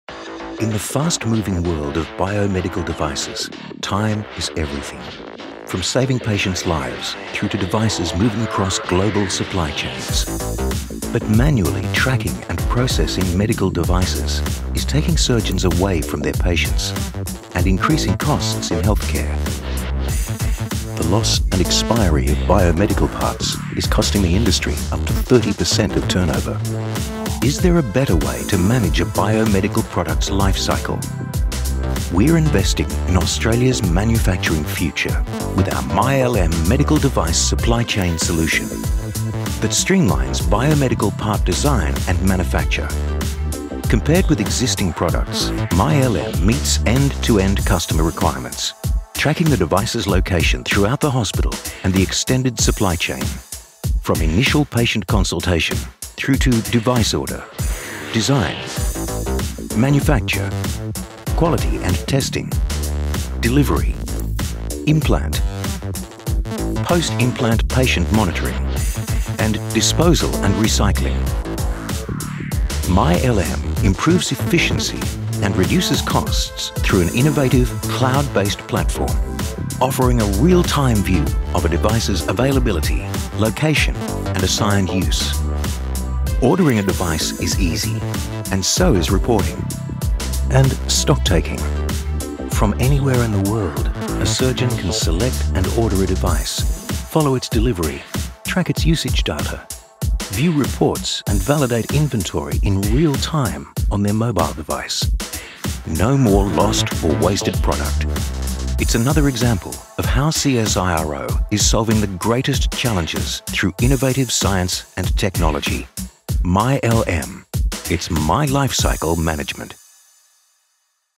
MyLM™ - It’s my lifecycle management
We’re investing in Australia’s manufacturing future with our MyLM™ medical device supply chain solution that streamlines biomedical part design and manufacture.